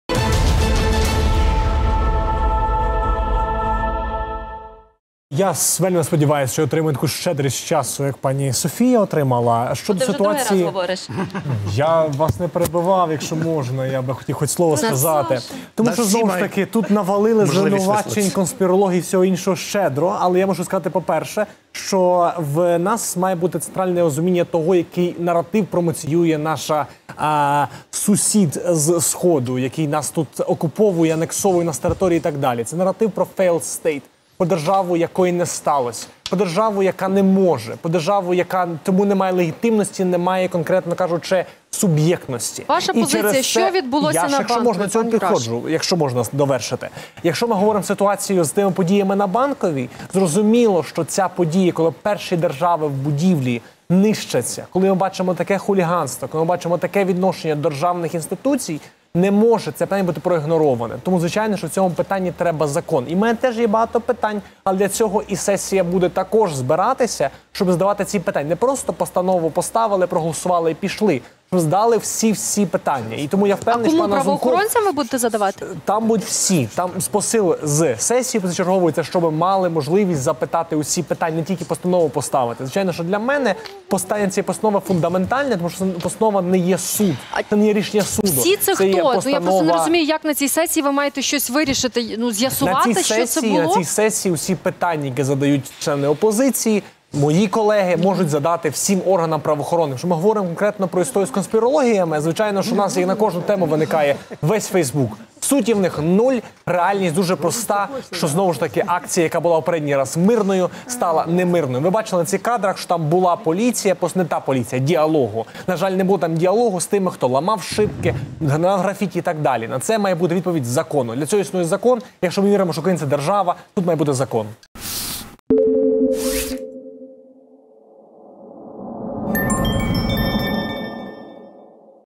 Юраш розповів, що на засіданні сесії Верховної ради розглянуть питання протестів на Банковій
«На цій сесії всі питання, які мають члени опозиції та мої колеги - зможуть поставити правоохоронним органам», - Святослав Юраш, народний депутат України від «Слуги народу» розповів, що на позачерговому пленарному засіданні сесії Верховної ради разом з правоохоронцями будуть обговорювати протести на Банковій.